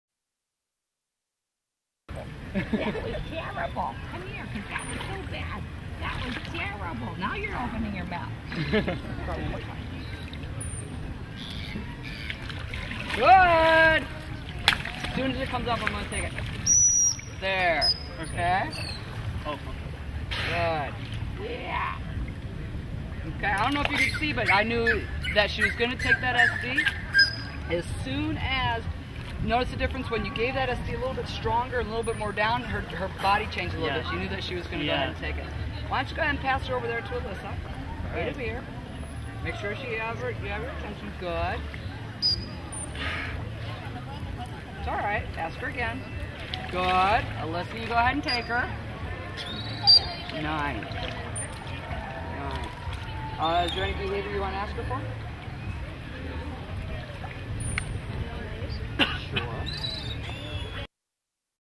students learn how to bridge a behavior
ABC Dolphin Trainer Academy students learn how to correctly bridge the tail wave.